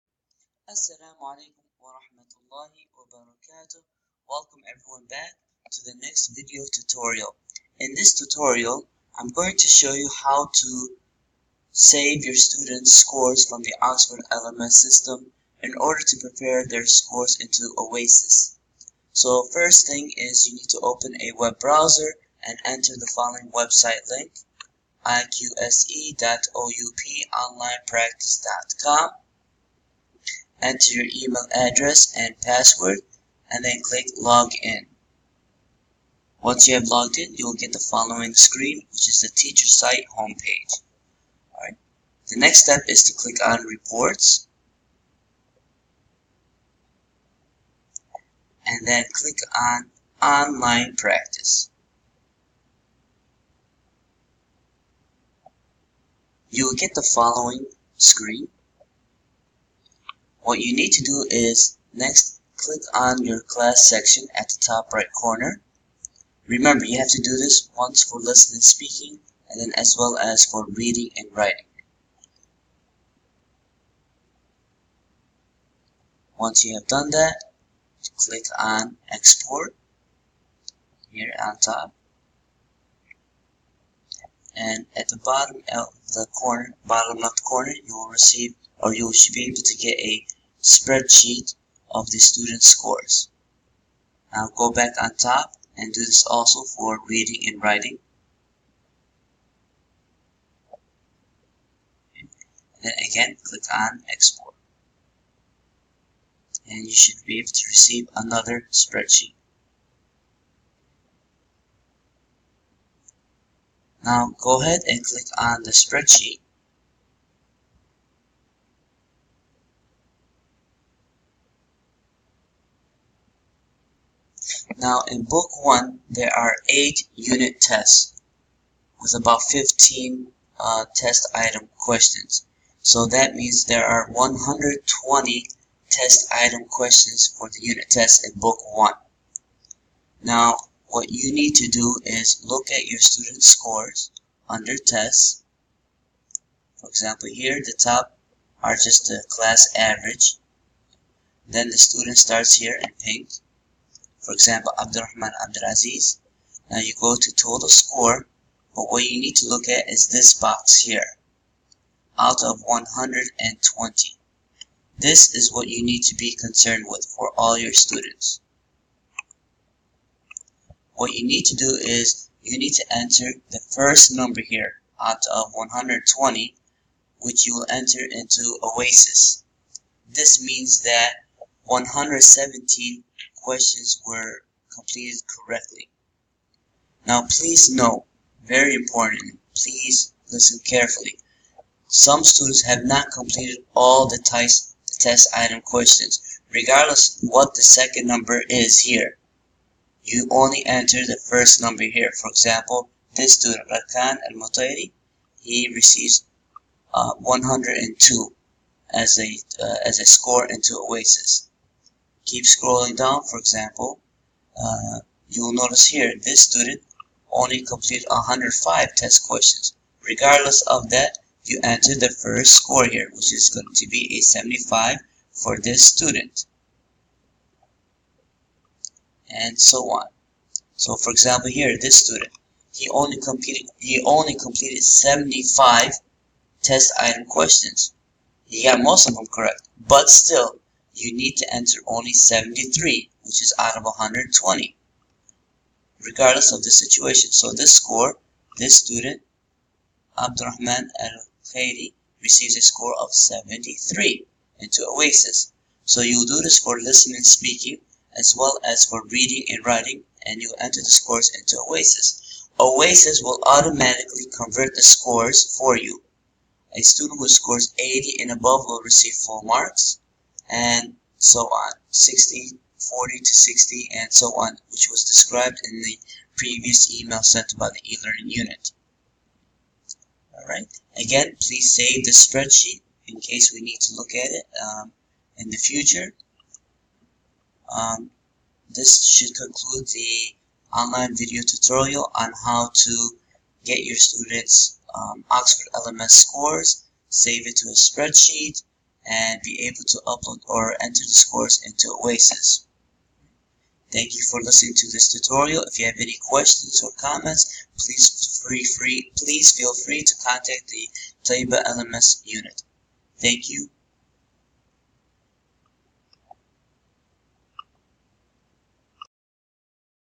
Getting StudentLMS Scores
This tutorial is for Taibah ELC teachers.  It shows instructions on how to get students lms scores from Oxford in order to enter them into OASIS.